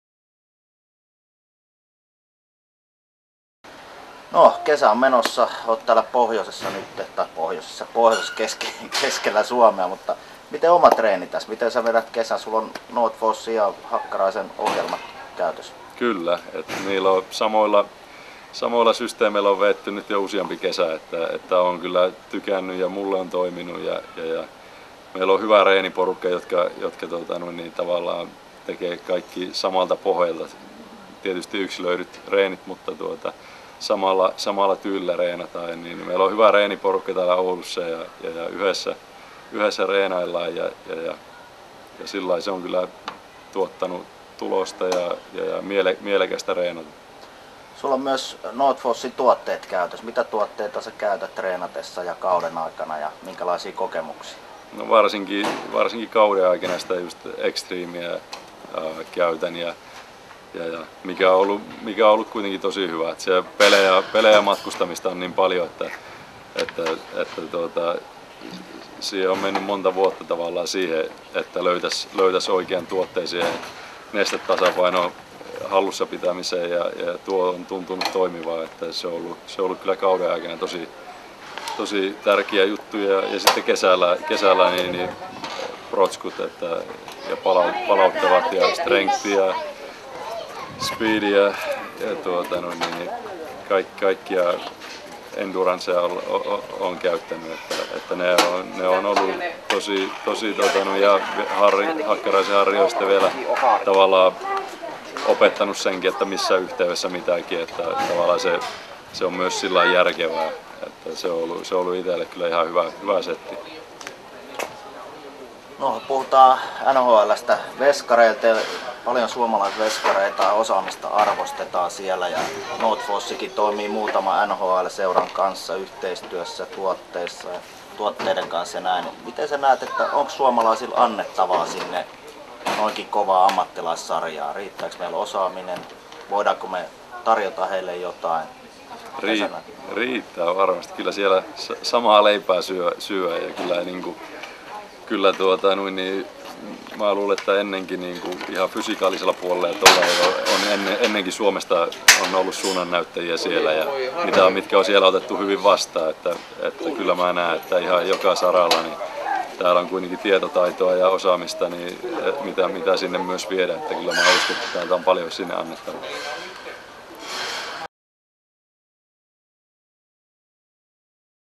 Pekka Rinne on huippumaalivahti, joka luottaa vain huipputuotteisiin.
Pekka Rinne luottaa Northforcen asiantuntemukseen ja kokee, että kansaivälistä huippuosaamista voi viedä jopa maailman kovimpaan lätkäsarjaan.